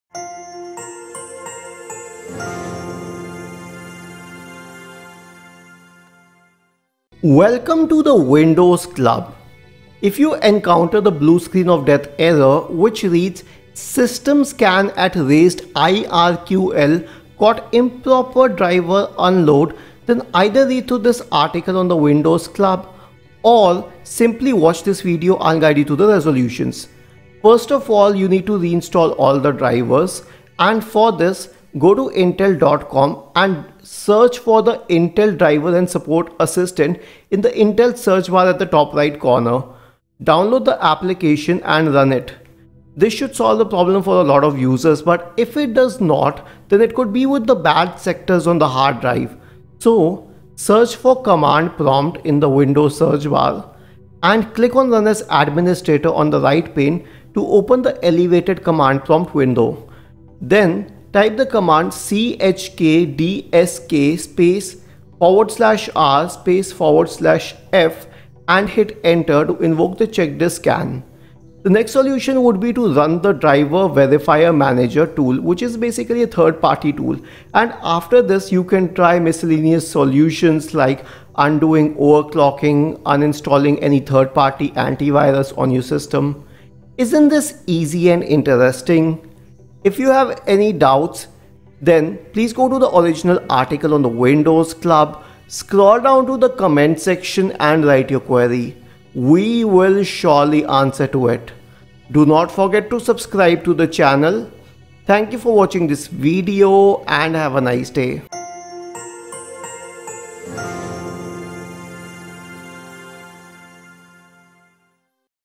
SYSTEM SCAN AT RAISED IRQL CAUGHT IMPROPER DRIVER UNLOAD
If after you perform a Windows 10 update or during an upgrade process and you encounter the SYSTEM SCAN AT RAISED IRQL CAUGHT IMPROPER DRIVER UNLOAD blue screen (BSOD) error, then this tutorial might help you.

Here are a few fixes and solutions:

1] Uninstall the problematic drivers

2] Download and install the latest Windows drivers

3] Run CHKDSK

4] Run Driver Verifier Manager

5] Undo Overclocking

6] Uninstall third-party antivirus/firewall

Timecodes:
0:00 Intro
0:27 Reinstall all the drivers
0:50 CHKDSK Scan
1:11 Run Driver Verifier Manager